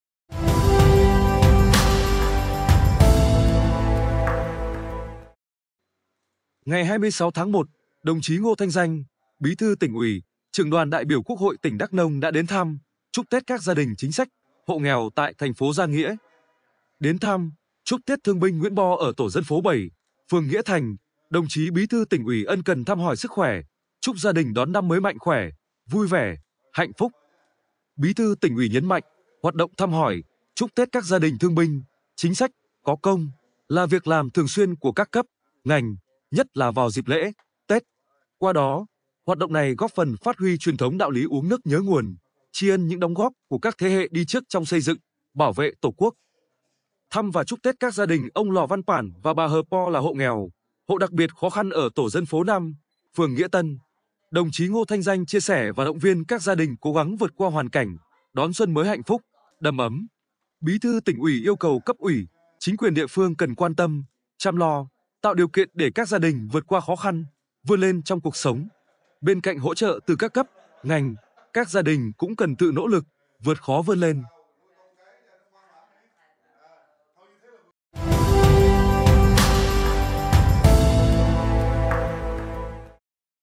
Đồng chí Ngô Thanh Danh thăm, tặng quà tết tại TP. Gia Nghĩa
Ngày 26/1, đồng chí Ngô Thanh Danh, Bí thư Tỉnh ủy, Trưởng Đoàn ĐBQH tỉnh Đắk Nông đã đến thăm, chúc tết các gia đình chính sách, hộ nghèo tại TP. Gia Nghĩa.